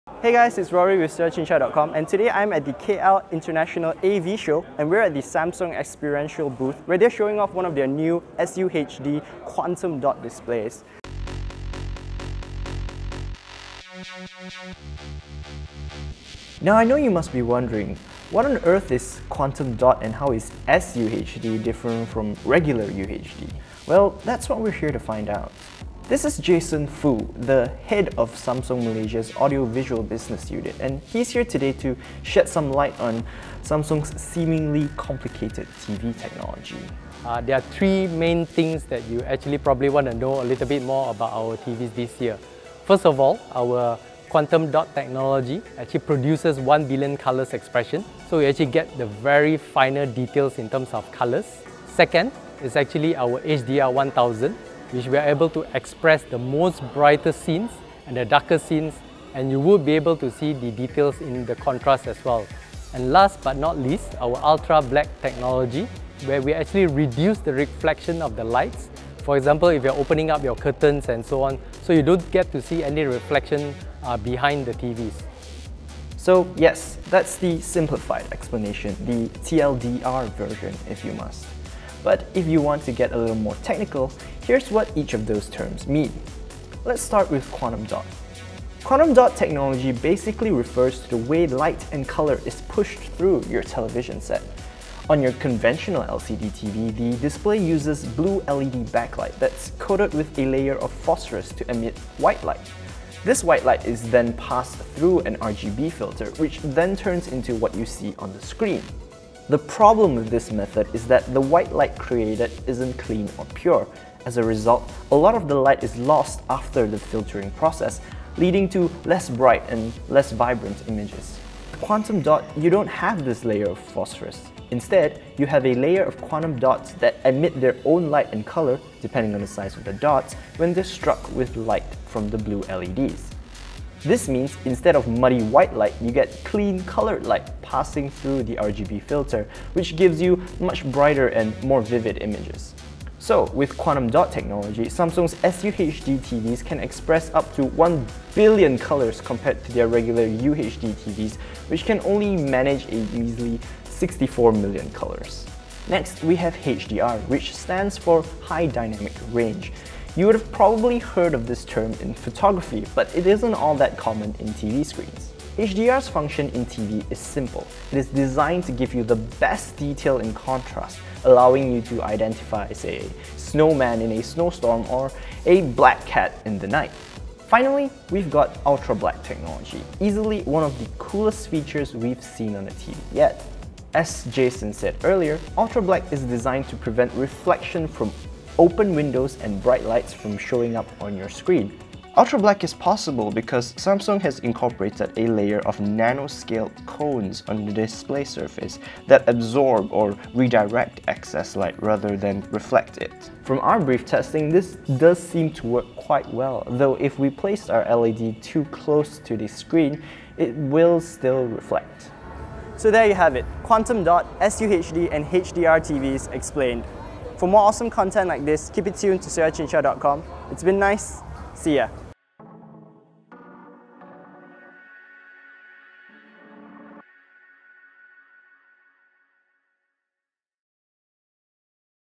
Samsung SUHD TV Quantum dot technology explained
What is new with Samsung's latest SUHD TV? And how is it different from other 4K TVs in the market. Watch Rory explains the difference with Quantum Dot, HDR 1000 and Ultra Black.